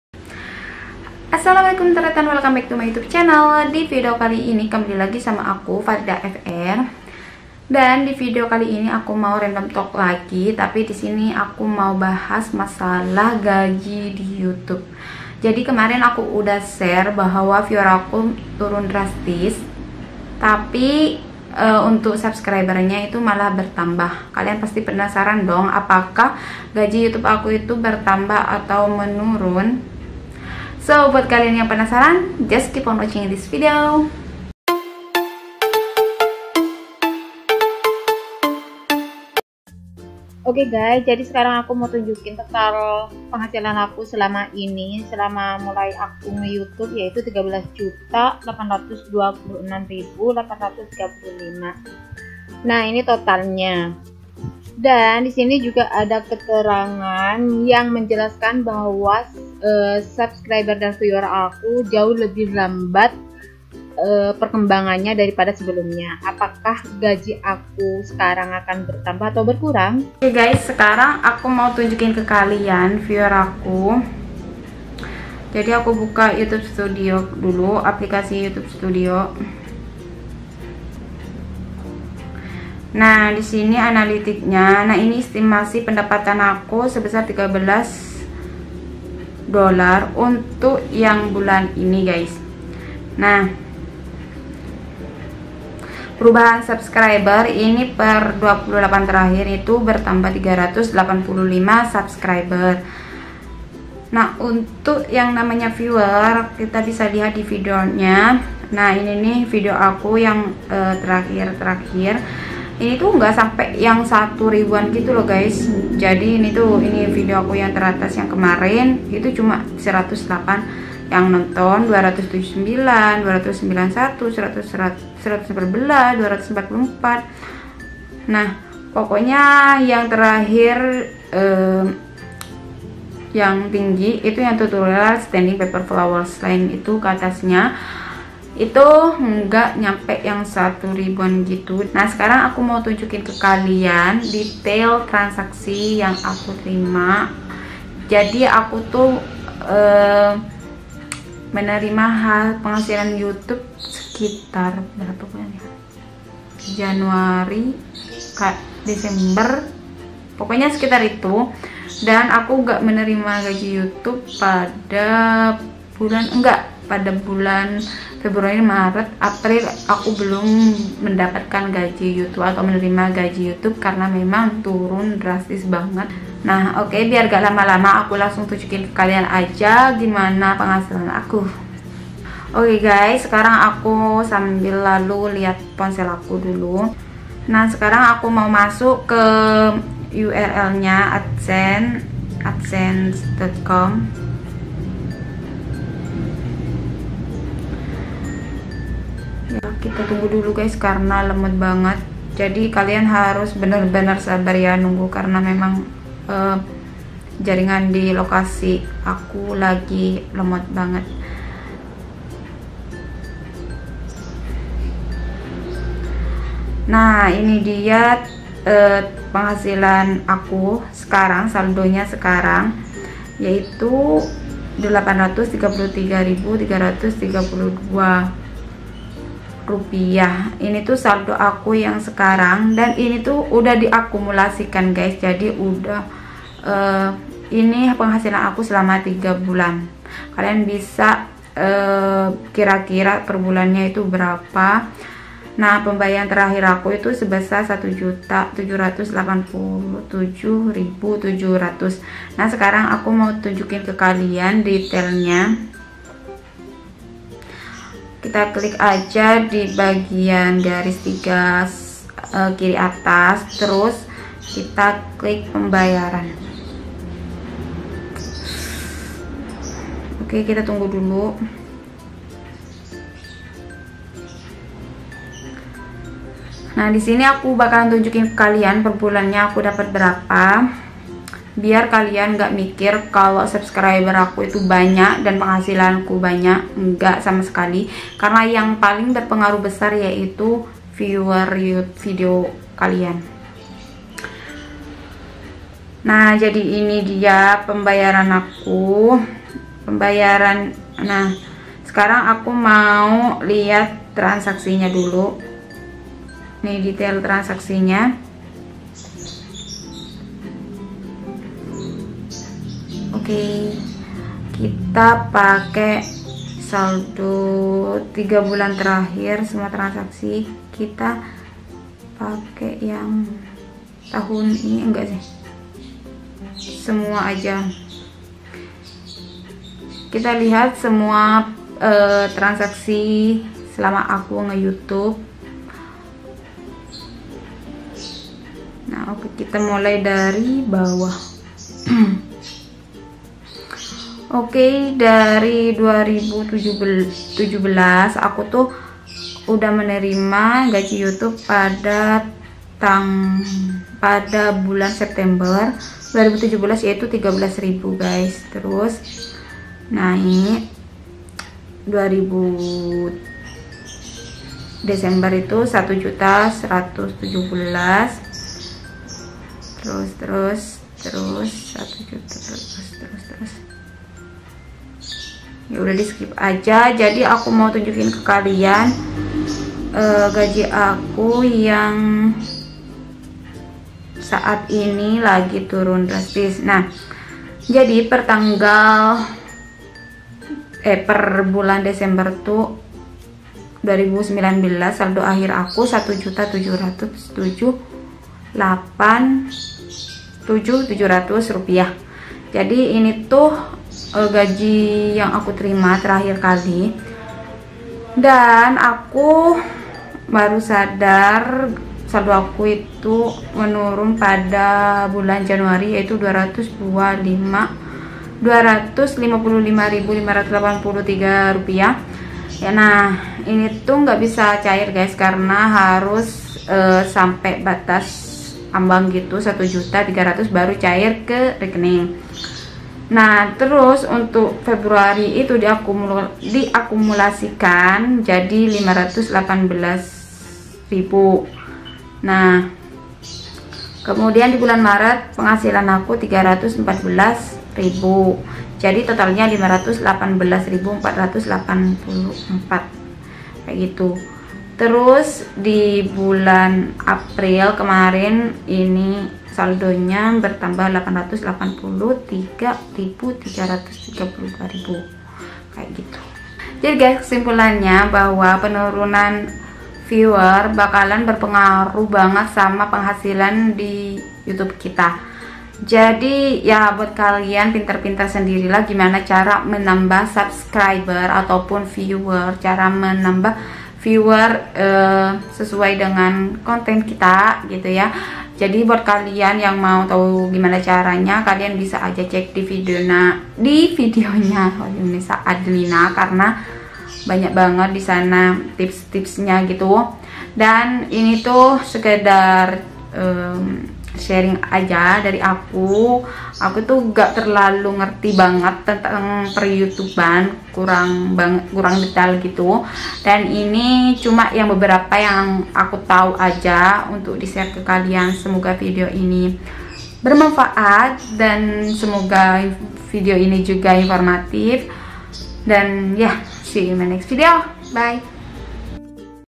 INILAH GAJI YOUTUBE DI TAHUN 2020 || Viewers Menurun Tapi Subscriber Bertambah? (no clickbait)
Apakah gaji youtube aku bwetambah atau menurun? ketika viewer aku menurun tapi subscribermeningkat.

EQUIPMENT:
Tonton Juga